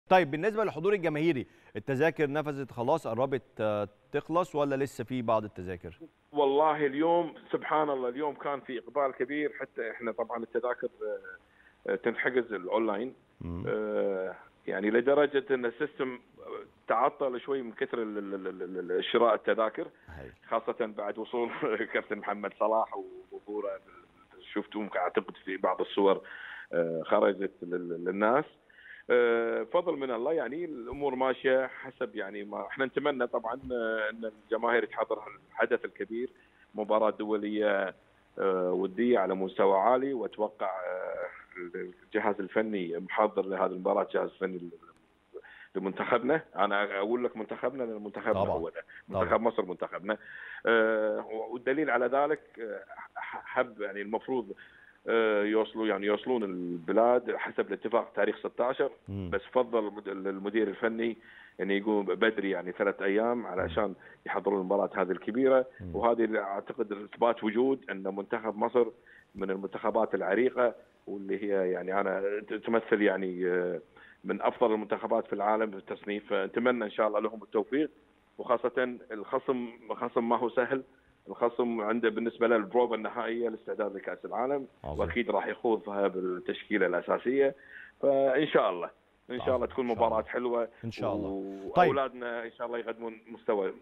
مؤيد شهاب يعلق على الاستقبال الحافل لمحمد صلاح: سيستم الحجز تعطل بعد وصول مو 💥
مؤيد شهاب رئيس اللجنة المنظمة لودية مصر وبلجيكا يعلق على الاستقبال الحافل لمحمد صلاح: سيستم الحجز تعطل بعد وصول مو 💥